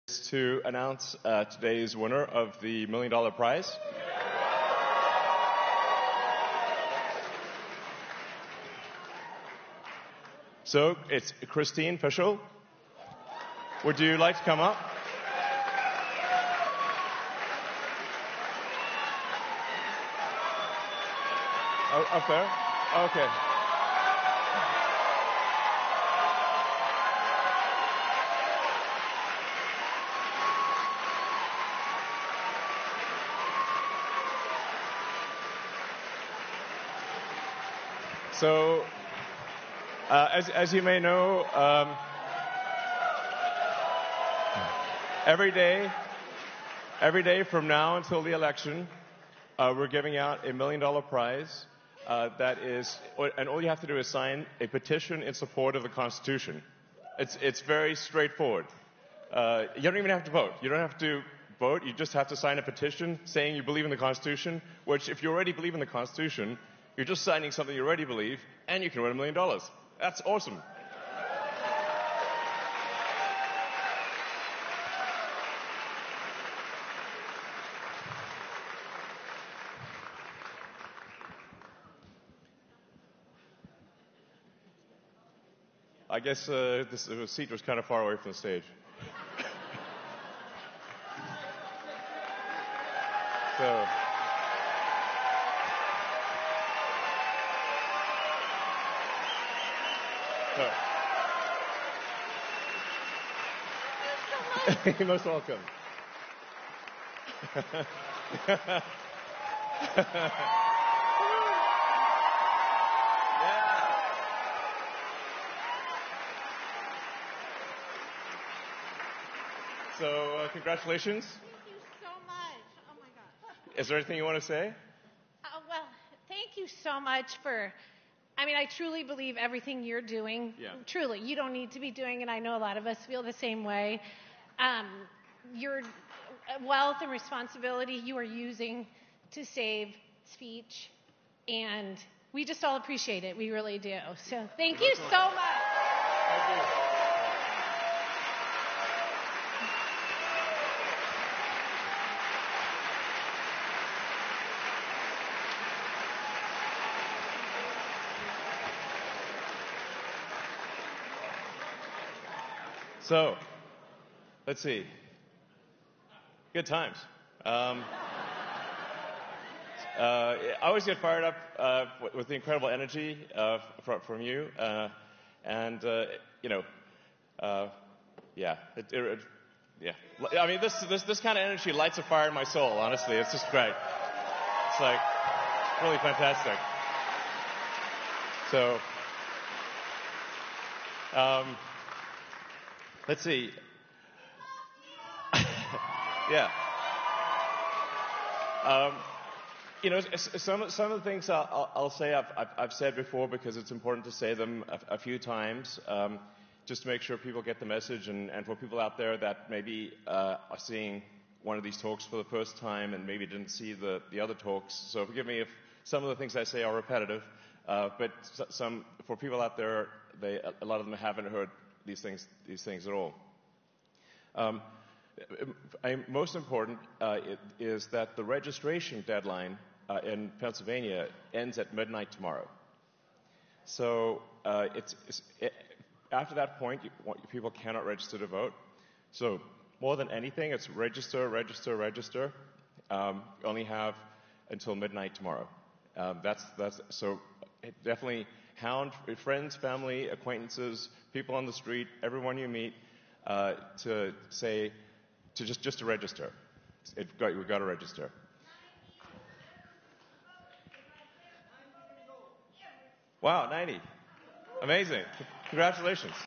Elon Musk Just Gave $1M to a Voter—Is He Doing More for Trump and Free Speech Than the GOP?
By giving the second $1M to the voter, Elon Musk is doing more for Trump, the Republican Party and is more effective fighting for Free speech than the leaders of the Republican Party.